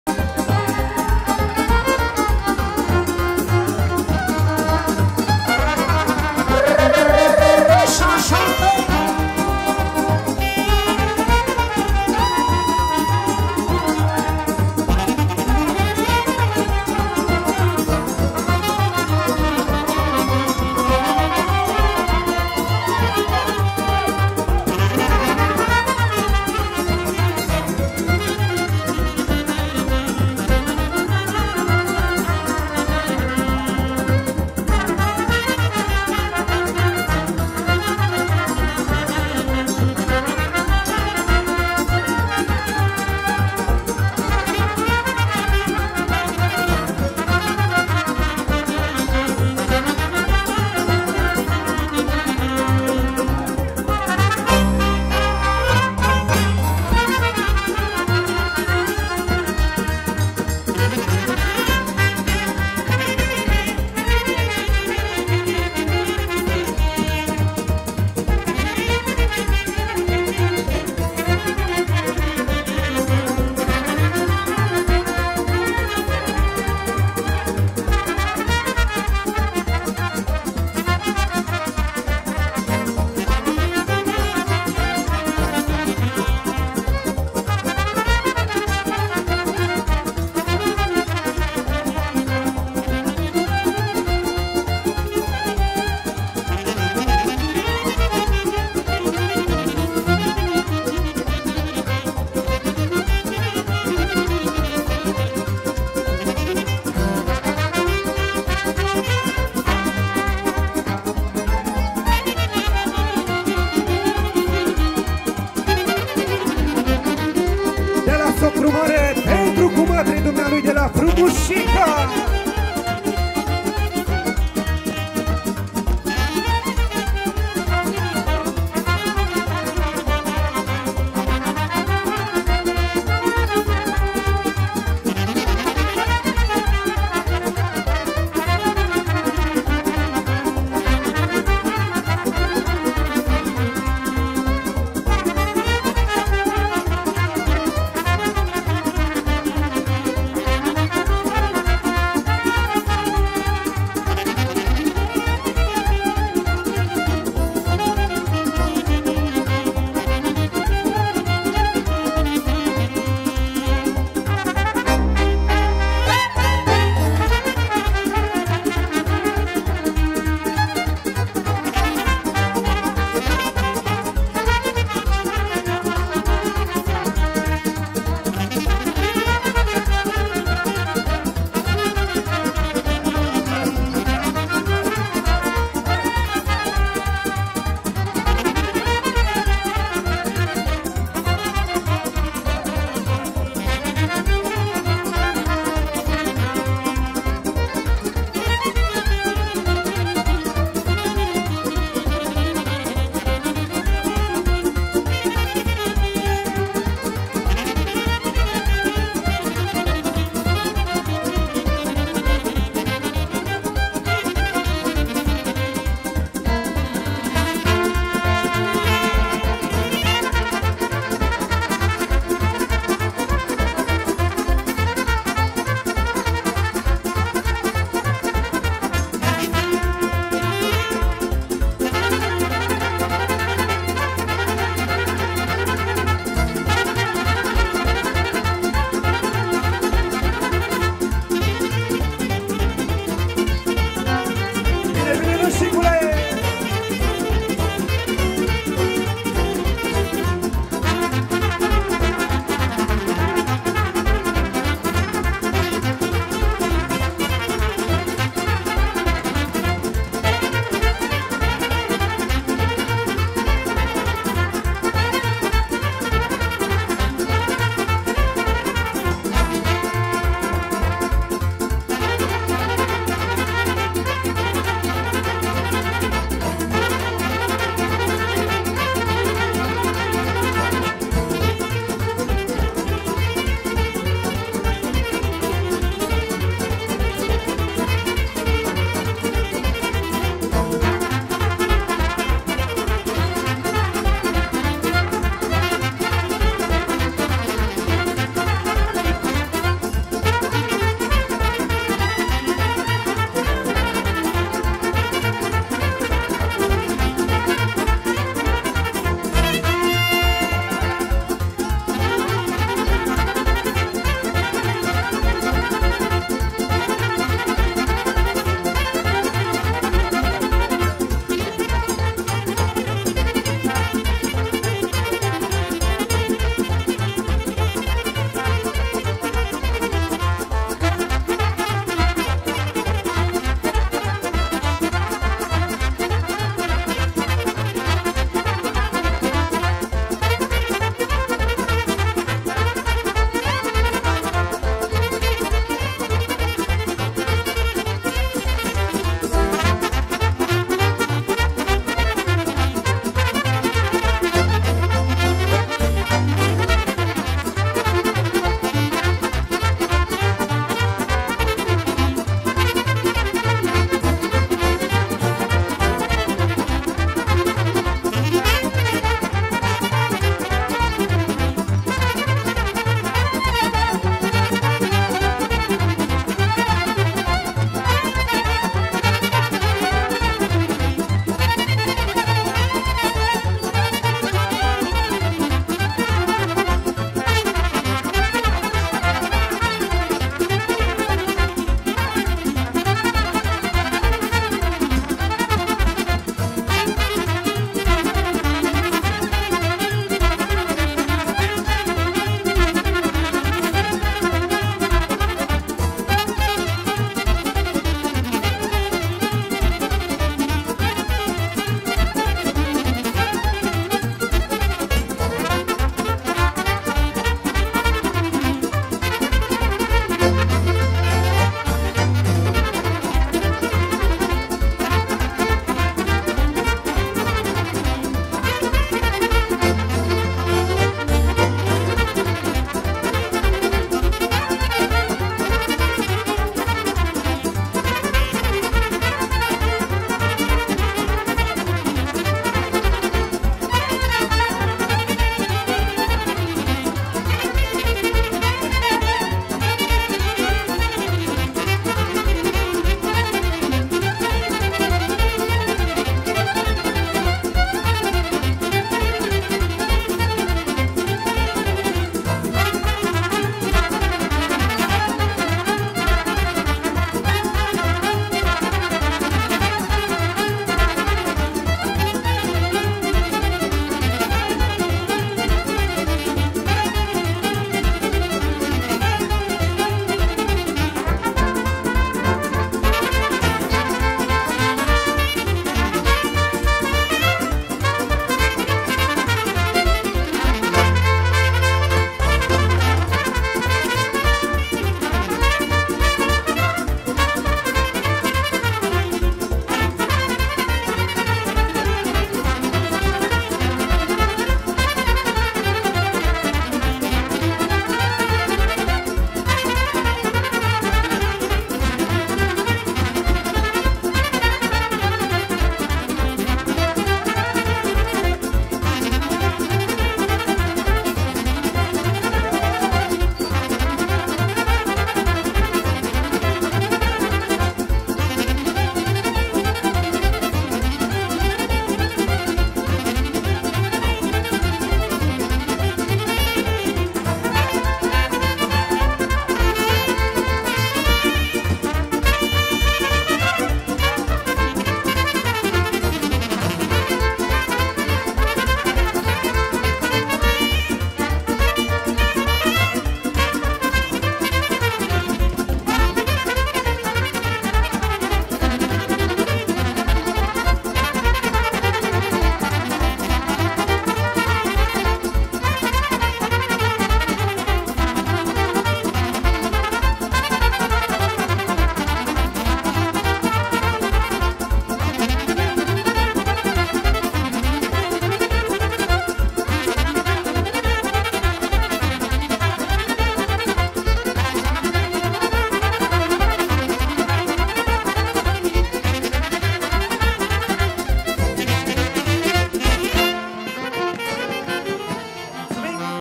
Colaj de Sarbe 💪🏽 Instrumentale Rapide - Cele mai frumoase melodii de petrecere
Formatia Zefir din Botosani este una dintre cele mai cunoscute formații de nunta din zona, care detine un stil unic si creeaza o atmosfera autentica petrecerilor si evenimentelor festive.
Video:@MISHAStudioRO
Formatia Zefir se distinge in acest colaj prin muzica sa de petrecere, care include sarbe, instrumentale rapide si muzica populara. Muzica de petrecere colaj oferita de formatia Zefir este un amestec perfect intre vechile melodii si stilurile muzicale moderne, creând o atmosfera unica. Pentru a aduce o experienta completa petrecerii, formatia Zefir se acompanieaza de vioara, toba, trompeta, saxofon si clape.